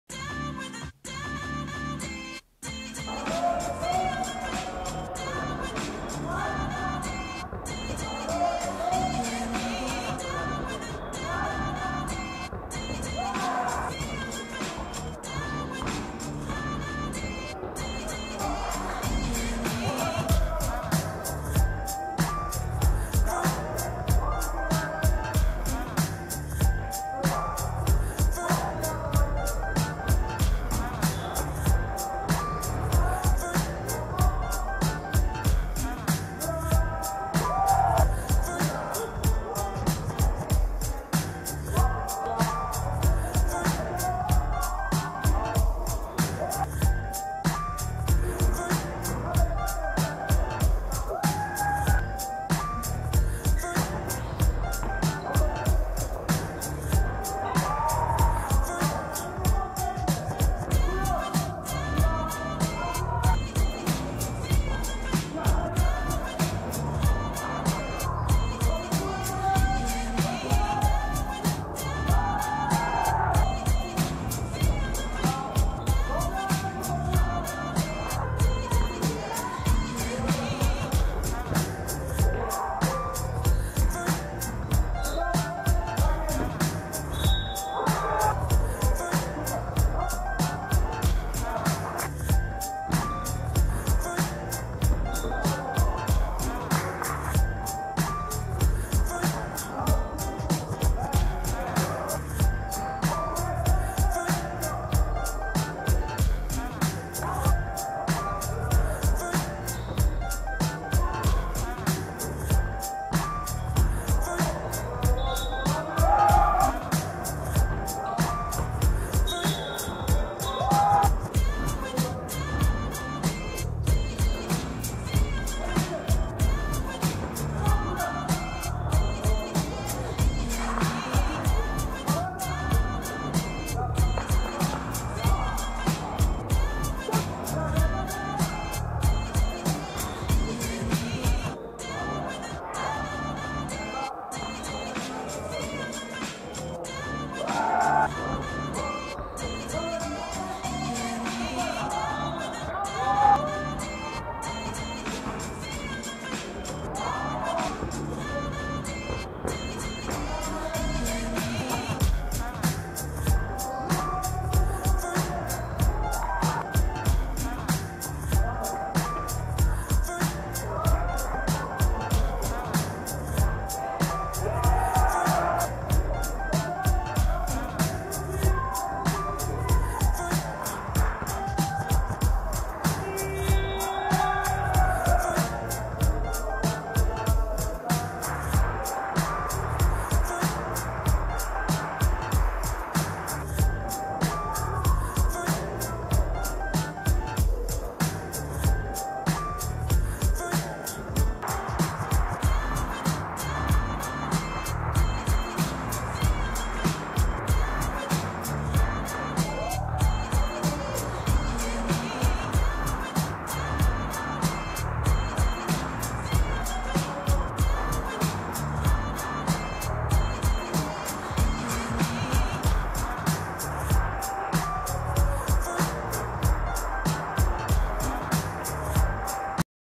10 12 FINAŁ Texas vs Kentucky – Liga Amerykańska 12
TEXAS 64:66 KENTUCKY
J.Tomaszewski 40pkt 7zb 3ast 7stl
A.Berntson 32pkt 42zb 4blk